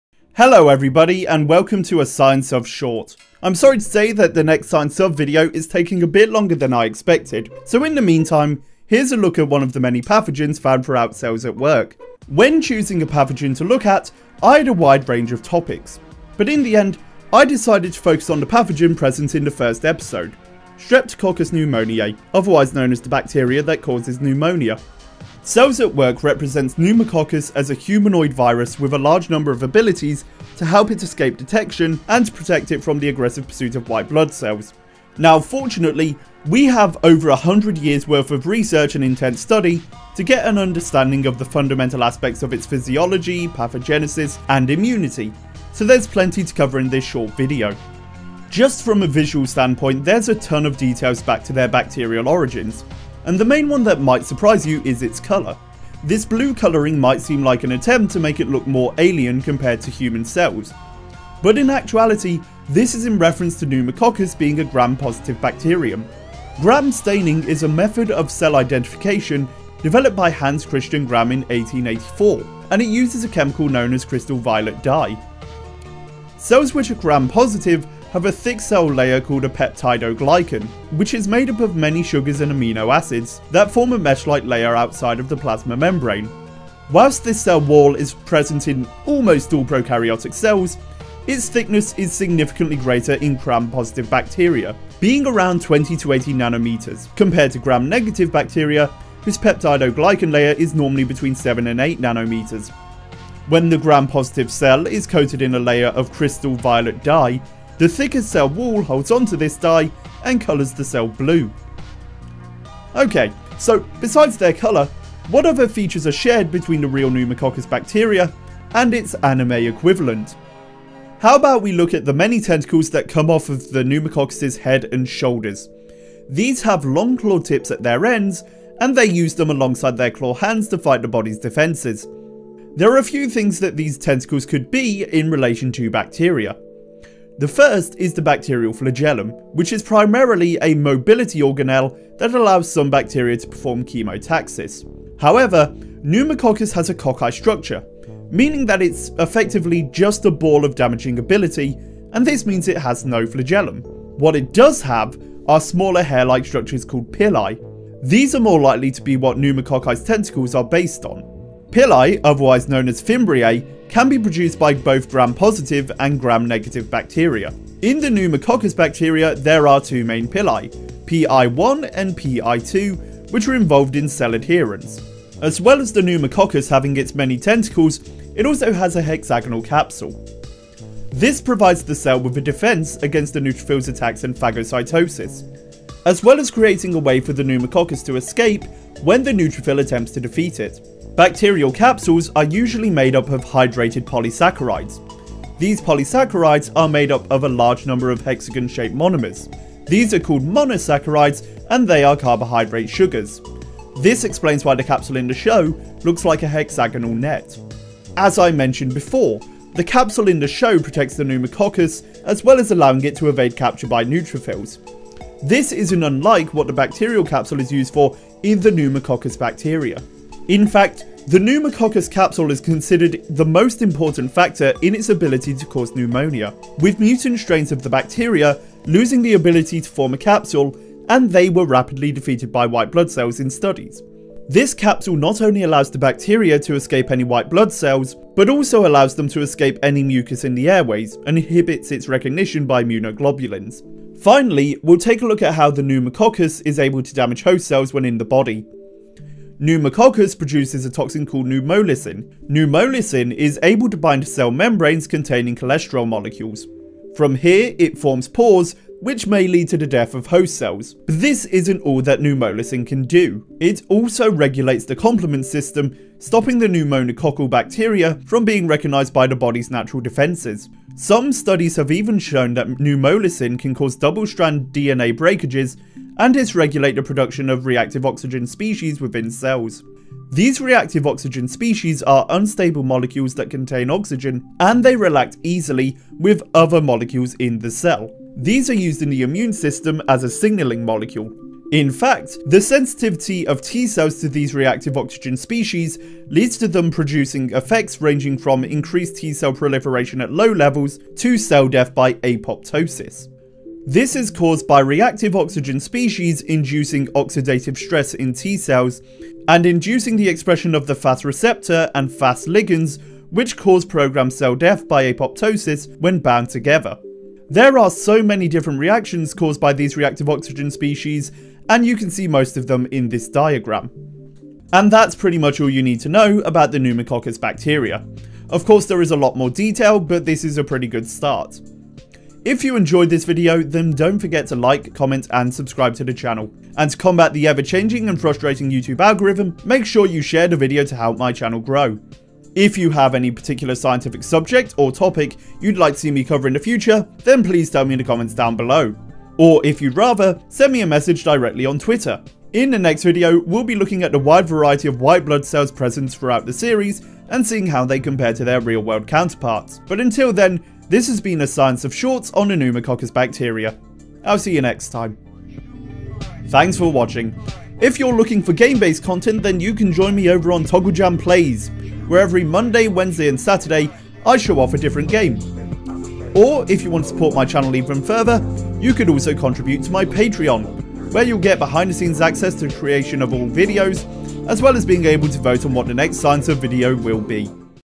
Big Blue Meanie - The Science of Cells At Work Hataraku Saibō - part 1.5
Continuing our series on cells at work we take a look at the first antagonist in the series, streptococcus pneumoniae otherwise known as the bacteria that can cause pneumonia.

Livestreams: Twitch.tv/togglejam

Cells at Work! is a Japanese manga series written and illustrated by Akane Shimizu. It features the anthropomorphized cells of a human body, with the two main protagonists being a red blood cell and a white blood cell she frequently encounters

Enjoy this video on Cells At Work?